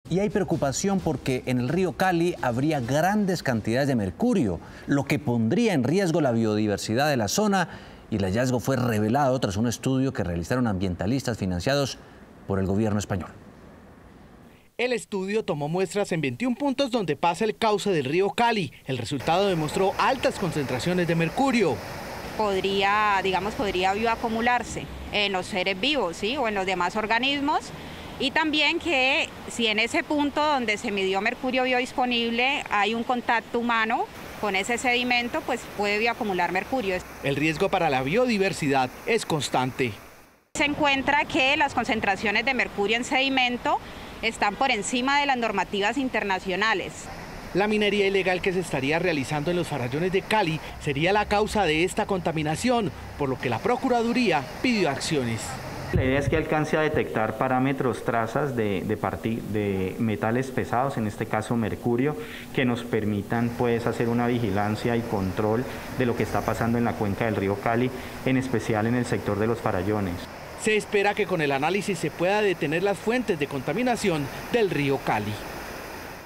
Encuentran grandes cantidades de mercurio en el río Cali
Un estudio realizado por ambientalistas y financiado por el gobierno español demostró altas concentraciones de mercurio en el cauce del río Cali. Se tomaron muestras en 21 puntos distintos.

“Podría acumularse en los seres vivos o en los demás organismos. Además, si en ese punto donde se midió mercurio hay un contacto humano con ese sedimento, pues puede bioacumular mercurio”, comenta Karen Vivas, investigadora ambiental.

La minería ilegal que se estaría realizando en los Farallones de Cali sería la causa de esta contaminación, por lo que la Procuraduría pidió acciones.